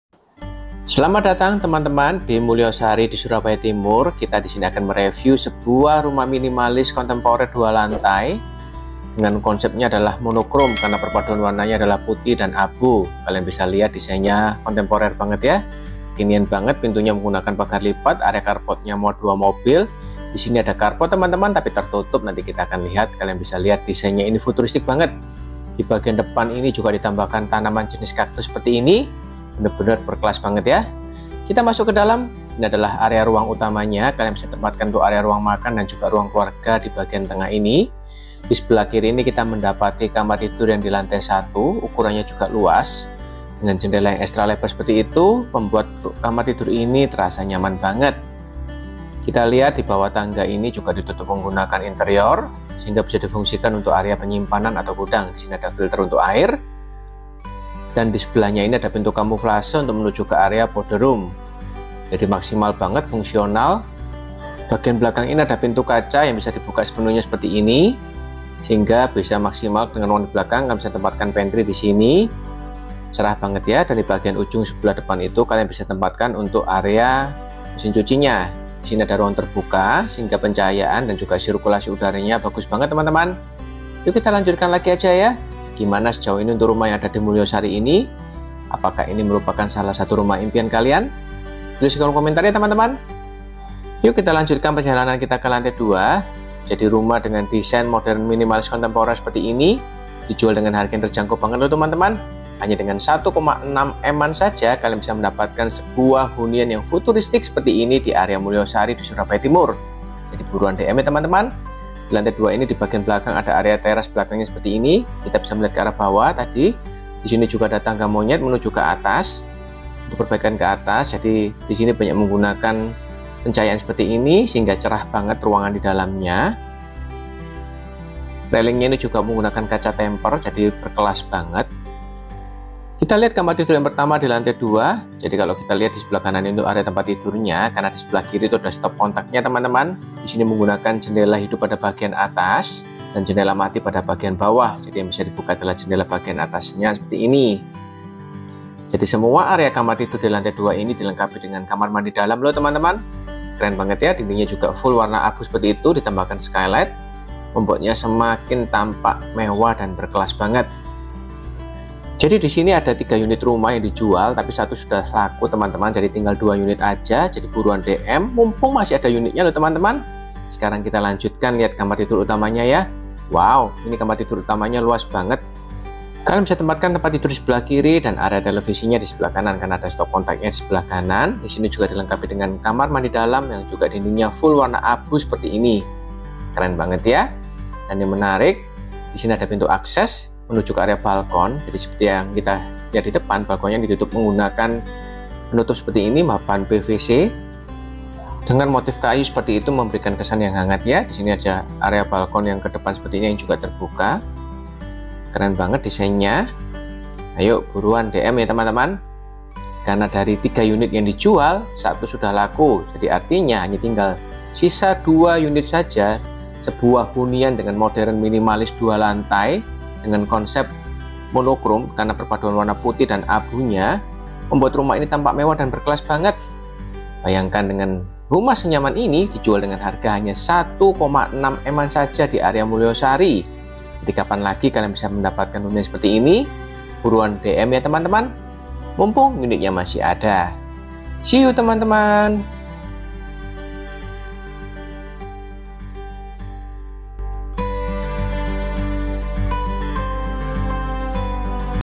SOLD !!! Review Rumah Monochrome Gaya Modern Minimalis Kontemporer Yang Berkelas di Mulyosari
Dijual Murah Rumah Monochrome Gaya Minimalis Kontemporer 2 Lantai di Mulyosari Surabaya Timur

● Dijual 3 Rumah Baru Gress (sisa 2)
● Lokasi 300 meter ke jalan Raya Mulyosari
● 2 Lantai
● Kualitas Premium
● Pondasi Strauss Pile kedalaman 12 Meter.

● LT 5.35 x 15 = 80m2
● LB 140m2
● SHM dan IMB
● 1  Kamar Lantai bawah                                  ● 2 Master Bedrooms
● 2 Carports
● Hadap : Utara
● Free Central Water Heater
● Free Tandon Atas Bawah
● Free pompa Atas Bawah
● listrik : 4400
● PDAM

Harga Jual : 1.68 M Nego